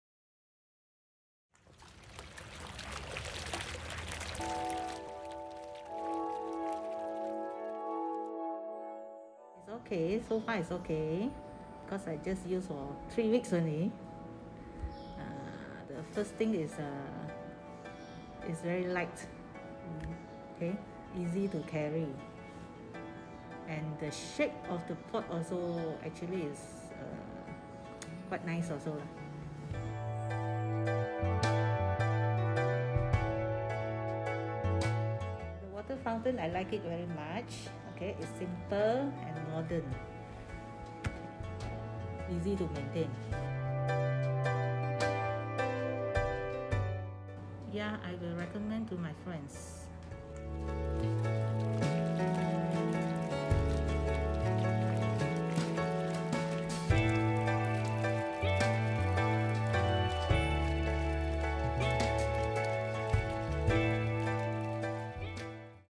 Customer Review: "The product is nice & modern, I like it" | Konzept Garden
Minimalist landscape design is a great way to create an outdoor space that's both functional and aesthetically pleasing.
By incorporating planter boxes and water fountains into your garden, you can create a space that's both beautiful and functional. Our product is the perfect way to achieve this look, and we're confident that you'll be satisfied with the results.
With our product, any garden can look amazing! So don't hesitate to give us a try. You won't be disappointed.